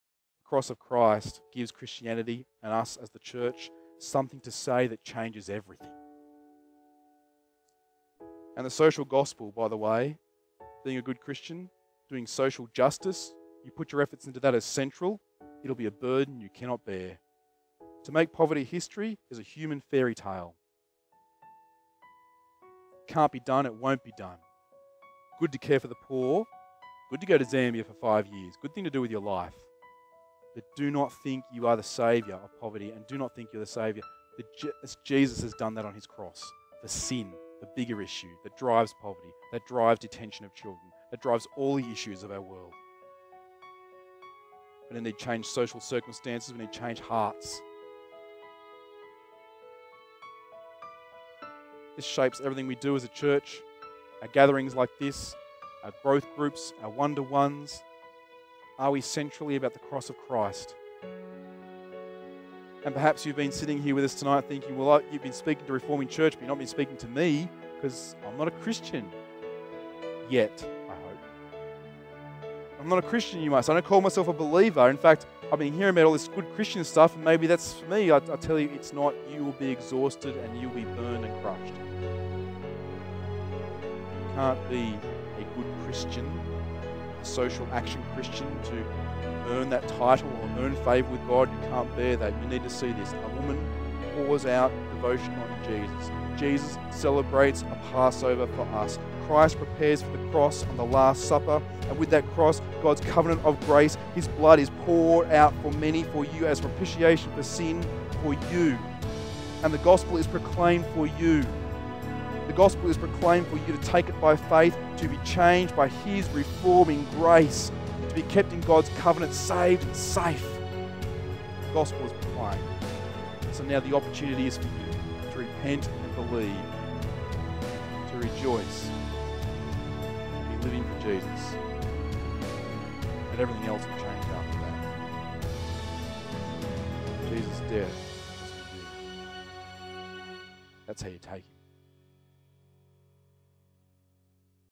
Social Justice Gospel
The Cross of Christ gives Christianity, and us as the Church, something to say that changes everything. And the Social Gospel, by the way, being a good Christian, doing social justice, if you put your efforts into that as central, it'll be a burden you cannot bear. To make poverty history is a human fairy tale.